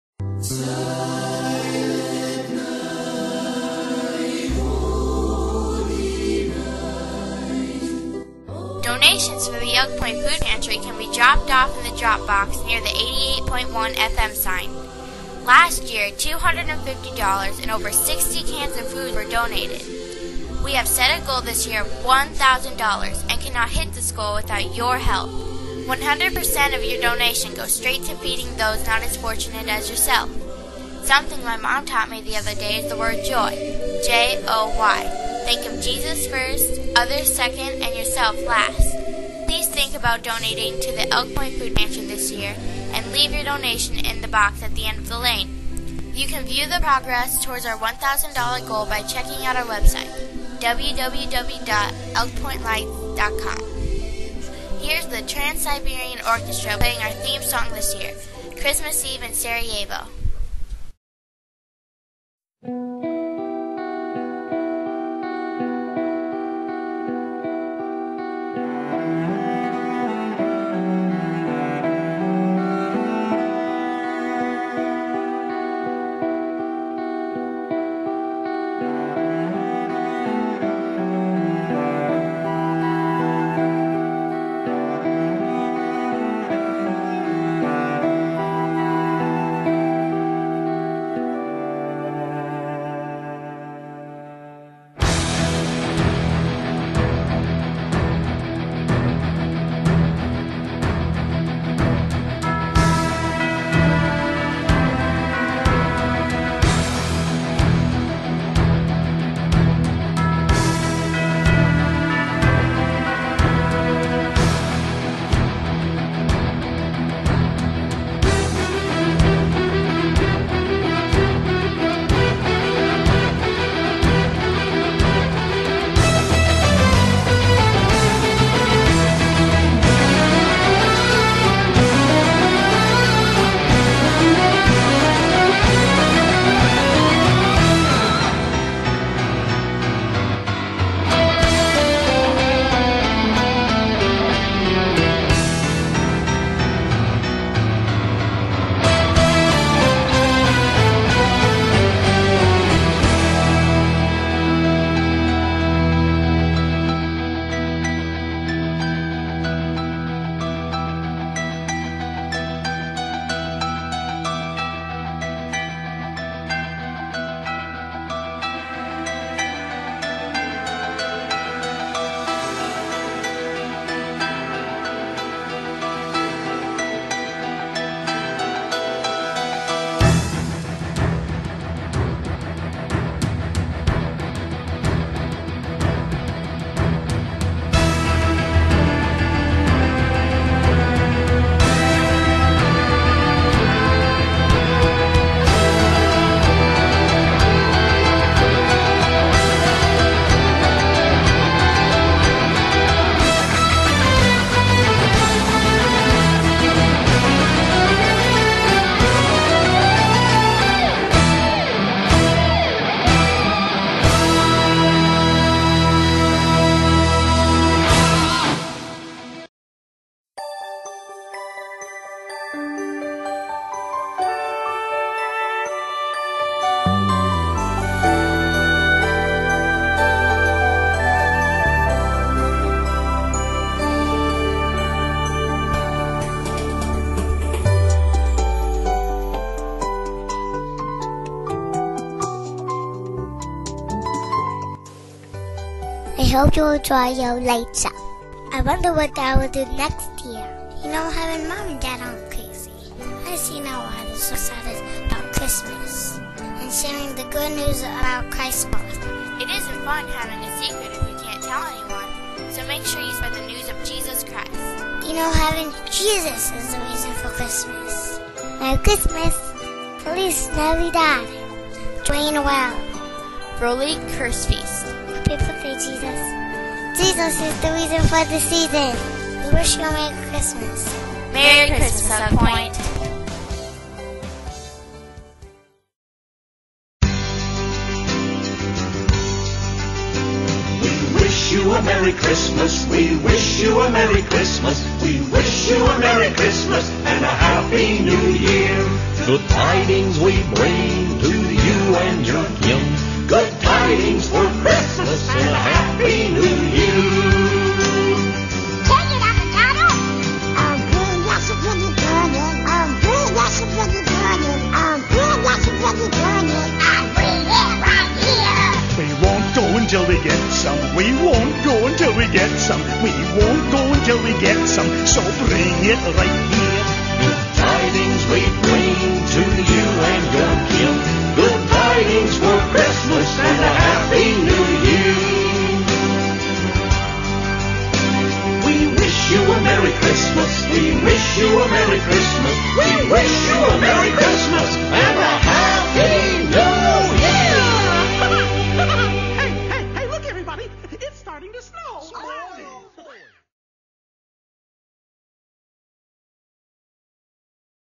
Elk Point Lights - 2nd Half Christmas 2007 Show
21,000 Lights computer animated with Christmas Music located in Elk Point, SD